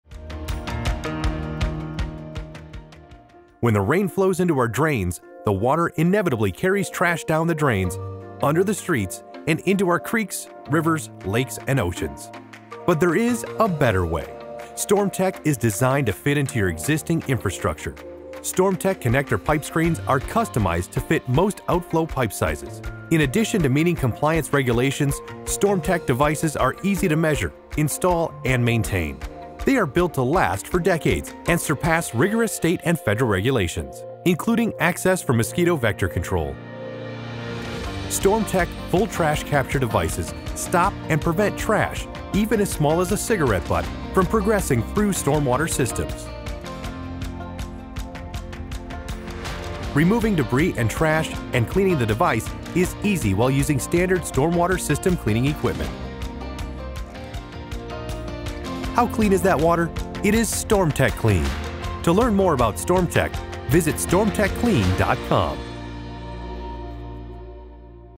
StormTek Full Trash Capture Device - how it works & what it does
When the rain flows into our drains, the water inevitably carries trash down the drains, under our streets and into our creeks, rivers, lakes, and oceans.

But there is a better way. StormTek is designed to fit into your existing infrastructure. StormTek connector pipe screens are customized to fit most outflow pipe sizes.

In addition to meeting compliance regulations, StortmTek devices are easy to measure, install and maintain. They are built to last for decades and surpass rigorous state and federal regulations, including access for mosquito vector control.

StormTek full trash capture devices stop and prevent trash, even as small as a cigarette butt, from progressing through stormwater systems.

Removing debris and trash, and cleaning the device is easy while using standard storm water system cleaning equipment.

How clean is that water? It is StormTek clean. To learn more about StormTek, visit StormTekClean.com